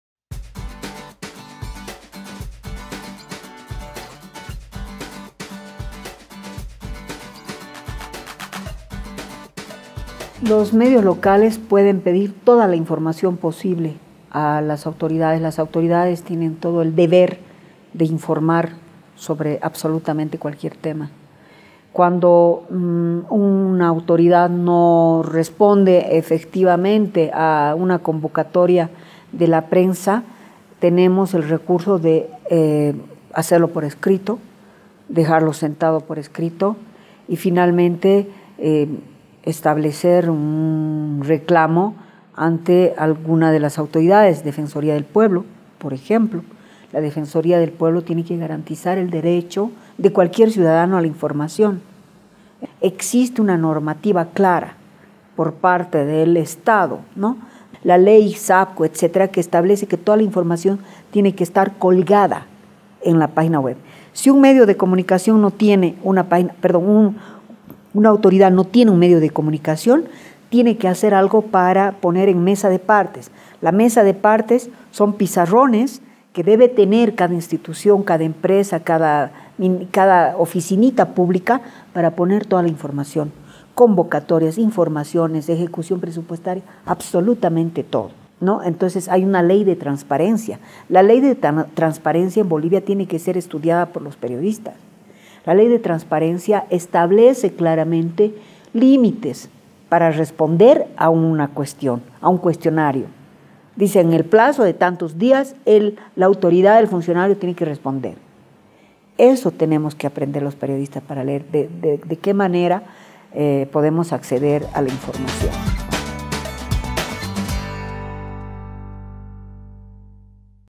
¿Cómo las y los periodistas pueden exigir sus derechos a la información?
"En este video la ex-ministra de comunicación Amanda Dávila explica que derechos tienen periodistas en Bolivia para exigir información y a base de que leyes pueden pedir infiormaciones y respuestas.

Este video es parte del ""Curso de especialización en periodismo radiofónico digital"" del Centro de producción radiofónica (CEPRA) para Radios Comunitarios y RPO.